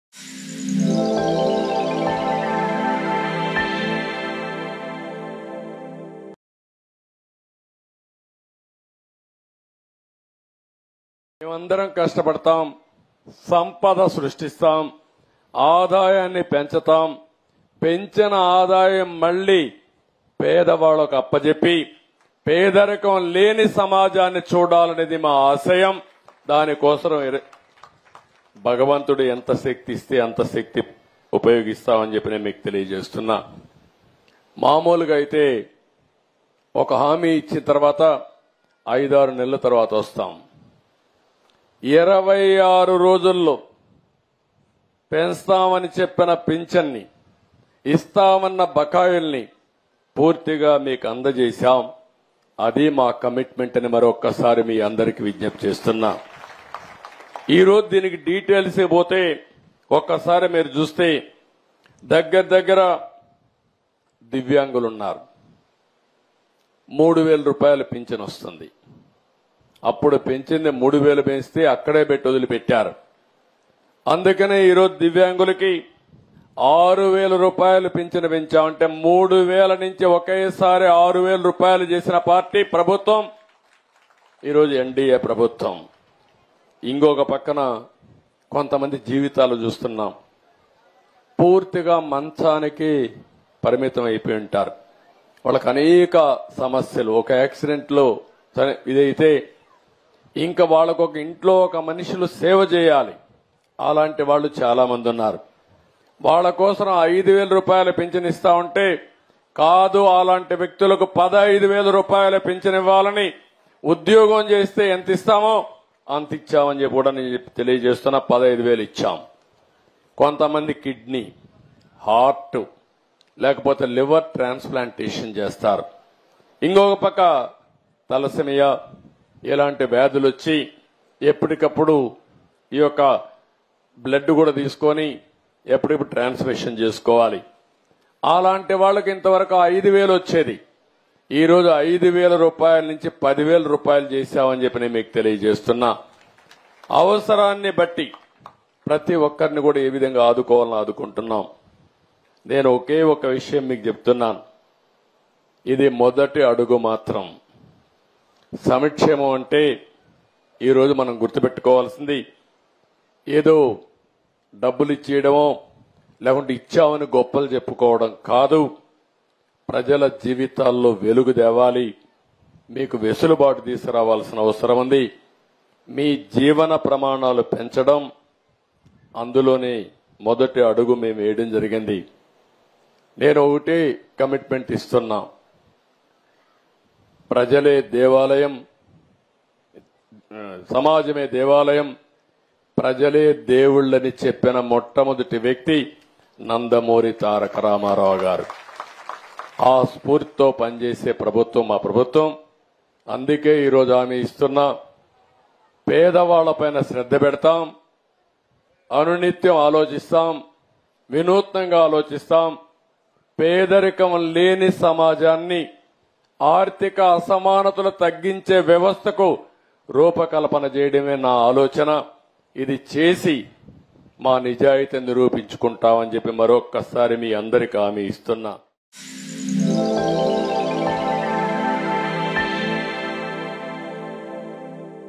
ప్రజల జీవితాల్లో వెలుగు నింపడమే నిజమైన సంక్షేమం. Our government works in the spirit of NTR - CM
అందరూ సమిష్టిగా  కలిసి పనిచేద్దాం. సంపద సృష్టిస్తాం.. ఆదాయం పెంచుతాం.. పెంచిన ఆదాయం పంచుతాం. ప్రజల జీవితాల్లో వెలుగు నింపడమే నిజమైన సంక్షేమం. ప్రజల జీవన ప్రమాణాల పెంపులో మొదటి అడుగు పడింది. సమాజమే దేవాలయం.. ప్రజలే దేవుళ్లని ఎన్టీఆర్ చెప్పారు.  ఎన్టీఆర్ స్ఫూర్తితో మా ప్రభుత్వం పనిచేస్తుంది. పేదలపై శ్రద్ధ పెడతాం.. అనునిత్యం వినూత్నంగా ఆలోచిస్తాం. ఆర్థిక అసమానతలు లేని సమాజం చూడాలనేదే నా ఆలోచన.